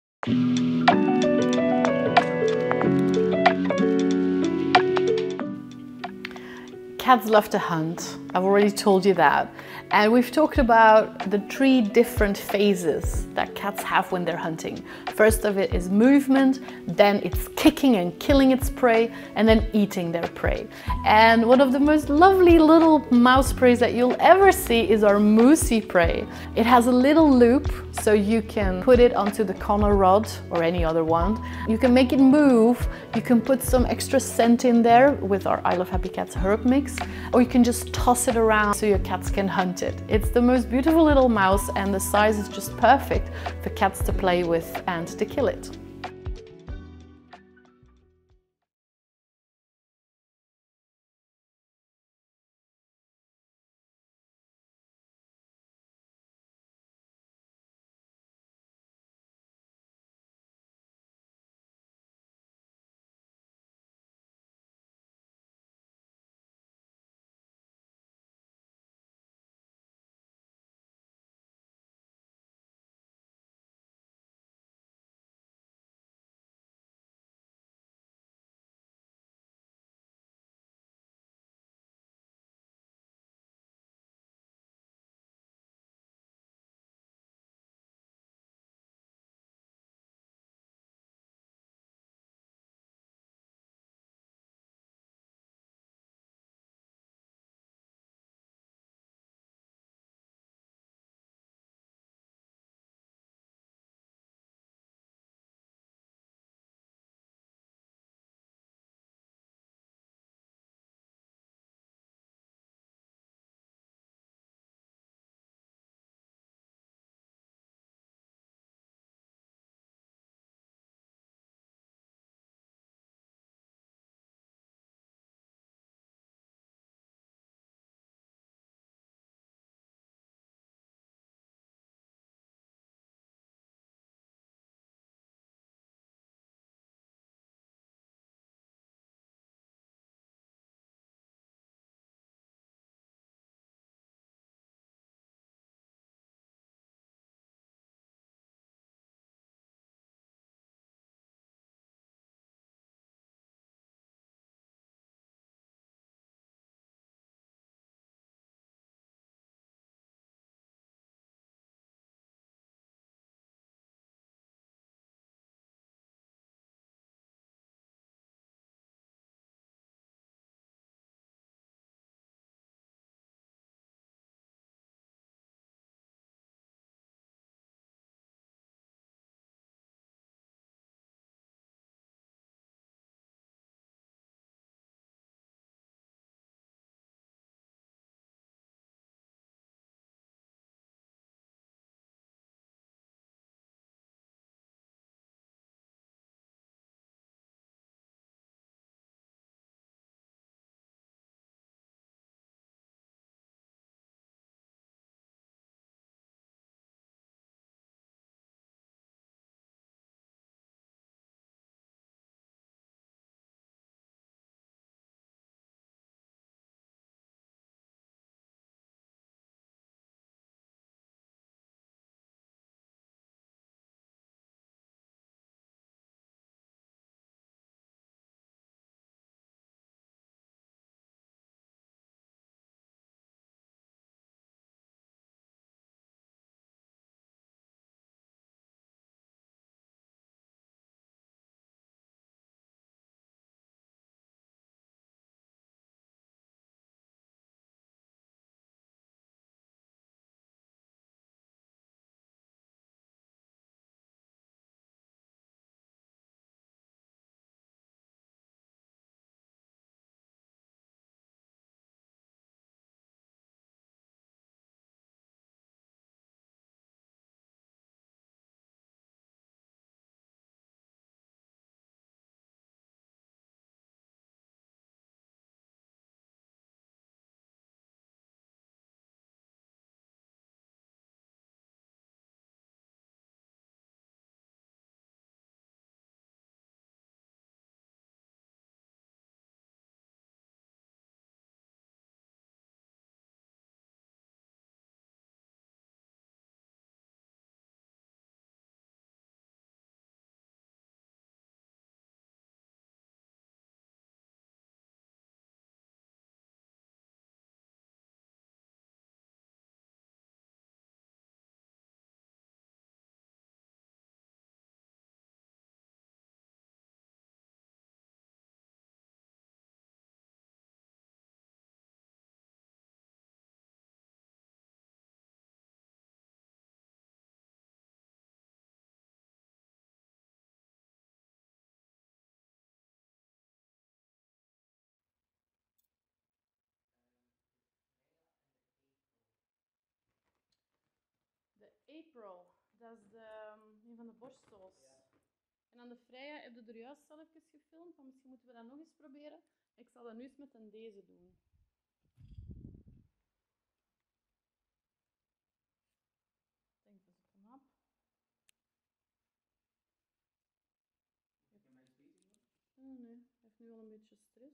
MOOSY – Jouet en forme de souris
MOOSY de D&D Home | I LOVE Happy Cats, est un petit jouet en forme de souris qui a été créé en collaboration avec Anneleen Bru, comportementaliste félin. Vous pouvez facilement attacher cette petite proie mignonne à la canne à pêche CONNOR pour jouer ensemble lors d’une partie de chasse interactive. Pour éviter que votre chat ne se lasse, remplacez la proie de temps en temps. Après la phase de la poursuite, détachez la souris de la canne pour faire réagir votre chat et qu’il libère les tensions. Le tissu intérieur du jouet MOOSY est fabriqué à l’aide d’une feuille craquelée. La garantie de nombreuses heures de jeu avec votre chat pour qu’il reste en forme et en bonne santé.

Conseil Happy Cats : Une fois le jeu terminé, mettez MOOSY à l’abri dans une boîte fermée. Ne le rendez à votre chat que quelques jours plus tard pour éviter qu’il se lasse de ses jouets qui traînent sur le sol. Ils sont déjà « morts » de toute façon ! 😊